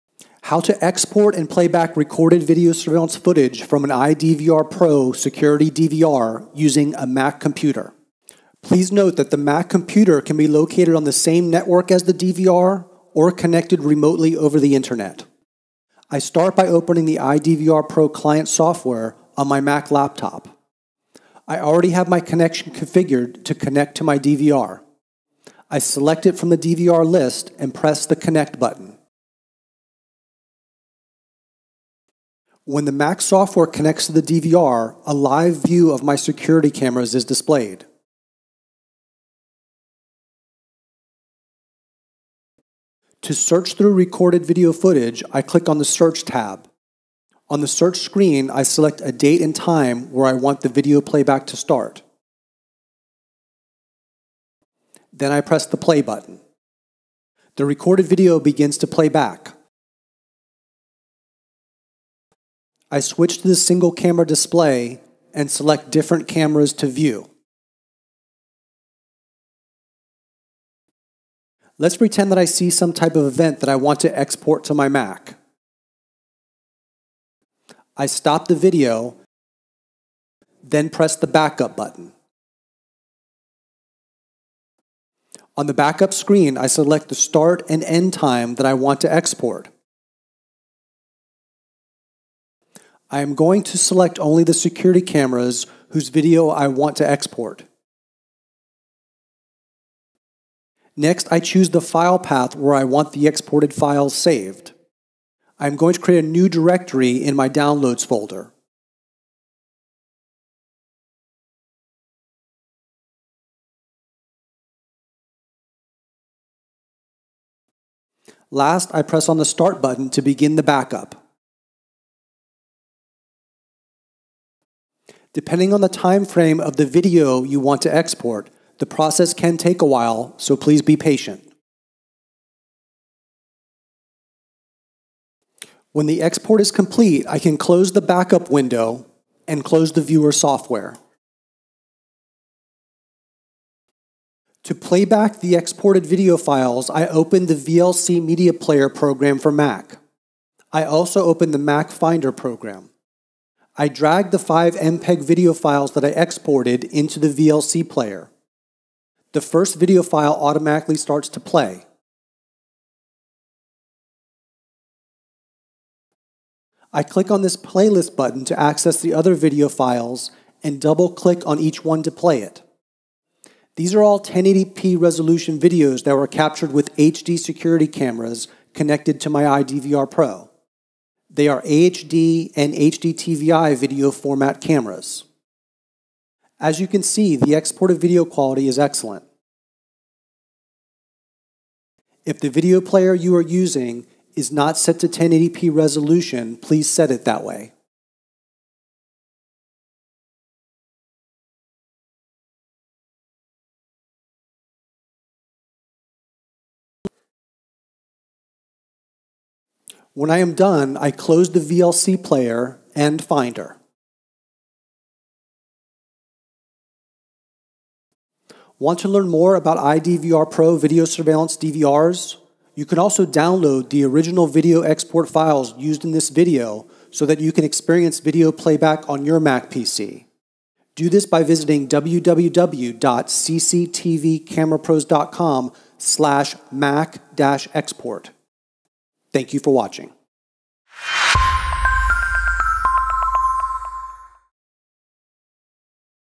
Recorded Video Surveillance Backup - iDVR-PRO HD CCTV DVR to Mac PC
- The Mac software for iDVR-PRO security camera DVRs makes it simple to export recorded video surveillance footage. Users can watch the video to follow these instructions on how to search video, export / back it up, then play it on their macintosh PC.

1) Users can use the Mac software to connect to their DVR remotely over the Internet or on the same network as the DVR.

2) Once connected, there is a search screen where the user can select a time to begin playing back the recorded video.

3) When an event is identified, the user can use the backup function to select the time-frame, security cameras, and file path of the video files that they want to export.

4) After the files are saved, users can use the open source and free VLC media player to open and playback the MPEG video files that were exported.

iDVR-PRO surveillance DVRs are compatible with analog CCTV cameras as well as the latest HD-over-Coax type security cameras (AHD and HD-TVI).

In addition to the  outstanding Mac software that is included, software for Windows PCs is also provided. Mobile apps for iPhone and Android are also free.

All of the client software and mobile apps support remote live security camera viewing as well as recorded DVR video playback.